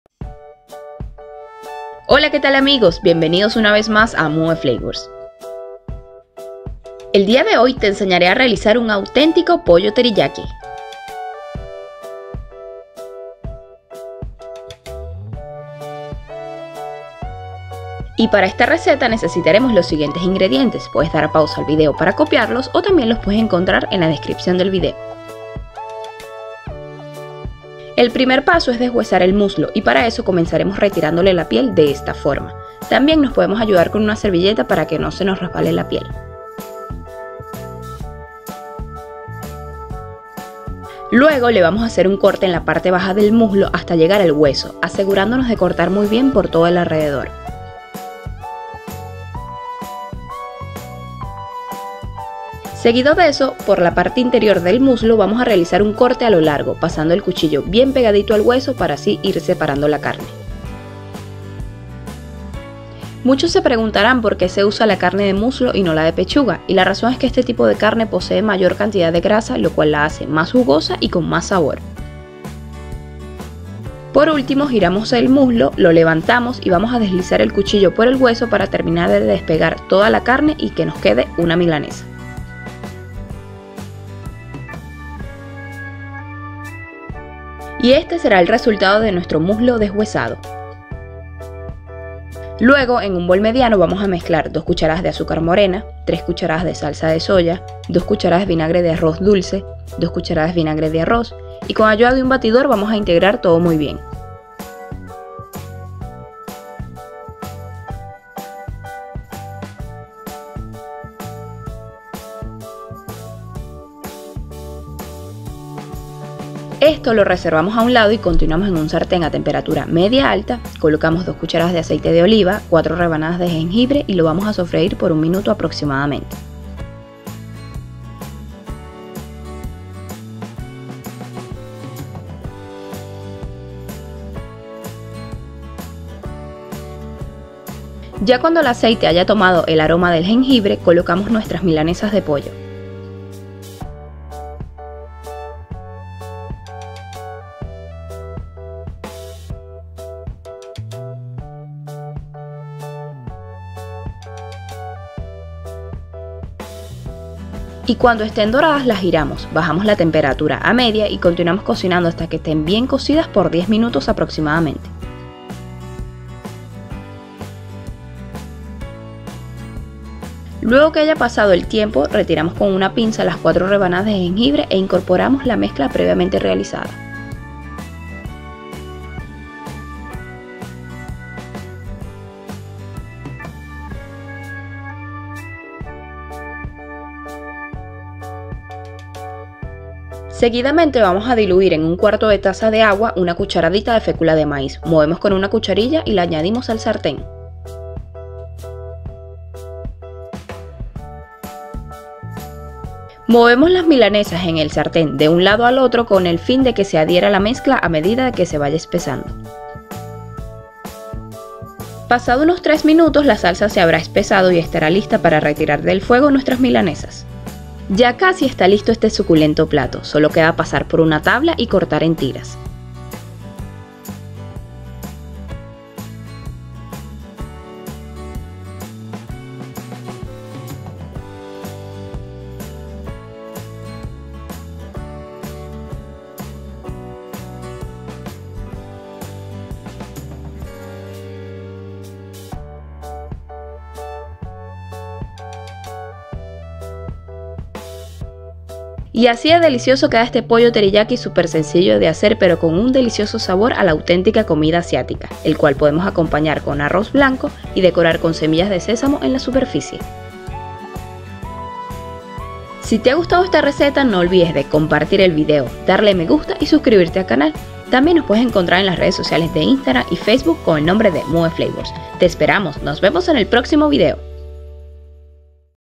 Pollo TERIYAKI fácil, rápido y delicioso | Chicken Teriyaki | Muve Flavors
La Comida asiática es conocida y amada alrededor del mundo y hoy te queremos enseñar a preparar un autentico POLLO TERIYAKI, con el mejor sabor y simplicidad y para elaborarla solo necesitas los siguientes ingredientes:

4 Muslos de pollos
2 Cdas de azúcar morena
3 Cdas de salsa de soya
2 Cdas de vinagre de arroz dulce
2 Cdas de vinagre de arroz
2 Cdas de aceite de oliva
4 Rebanadas de jengibre
1/4 taza de agua
1 Cdita de fécula de maíz (maicena)
1 Cdita de semillas de sémamo (ajonjolí)

y si te ha gustado nuestra receta considera subscribirte al canal